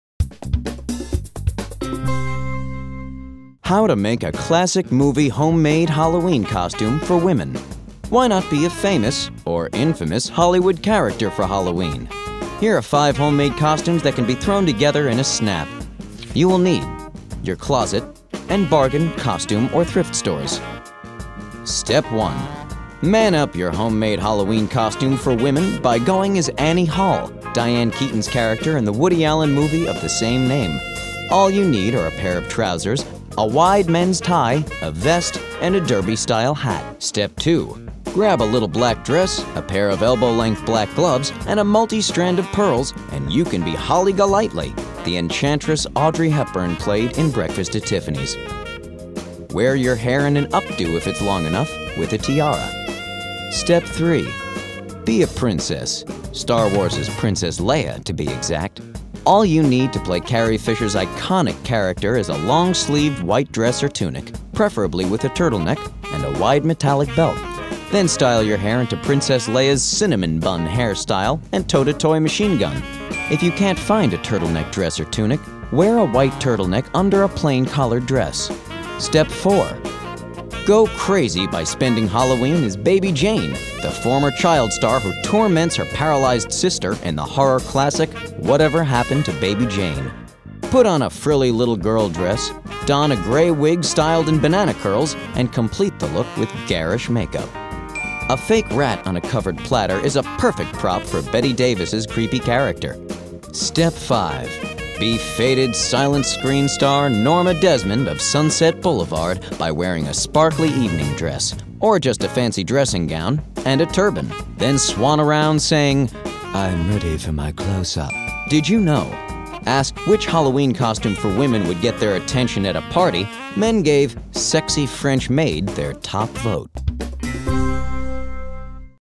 How to Make a Classic Movie Homemade Halloween Costume for Women
Why not be a famous -- or infamous -- Hollywood character for Halloween? Here are 5 homemade costumes that can be thrown together in a snap.

Step 1: Man up
Man up your homemade Halloween costume for women by going as Annie Hall, Diane Keaton's character in the Woody Allen movie of the same name. All you need are a pair of trousers, a button-down men's shirt, a wide men's tie, a vest, and a derby-style hat.

Step 2: Go lightly
Grab a little black dress, a pair of elbow-length black gloves, and a multi-strand of pearls, and you can be Holly Golightly, the enchantress Audrey Hepburn played in Breakfast at Tiffany's. Wear your hair in an updo if it's long enough, with a tiara.

Step 3: Be a princess
Be a princess -- Star Wars's Princess Leia to be exact. All you need to play Carrie Fisher's iconic character is a long-sleeved white dress or tunic, preferably with a turtleneck, and a wide metallic belt. Then style your hair into Princess Leia's "cinnamon bun" hairstyle and tote a toy machine gun.

Tip
If you can't find a turtleneck dress or tunic, wear a white turtleneck under a plain-collared dress.

Step 4: Go crazy
Go crazy by spending Halloween as Baby Jane, the former child star who torments her paralyzed sister in the horror classic, Whatever Happened to Baby Jane? Put on a frilly little-girl dress; don a gray wig styled in banana curls; and complete the look with garish makeup.

Tip
A fake rat on a covered platter is a perfect prop for Bette Davis's creepy character.

Step 5: Be a diva
Be faded silent screen star Norma Desmond of Sunset Boulevard by wearing a sparkly evening dress -- or just a fancy dressing gown -- and a turban. To complete this homemade Halloween costume, slap on lots of makeup, including false eyelashes, then swan around saying, "I'm ready for my close-up!"

Tip
Asked which Halloween costume for women would get their attention at a party, men gave "sexy French maid" their top vote.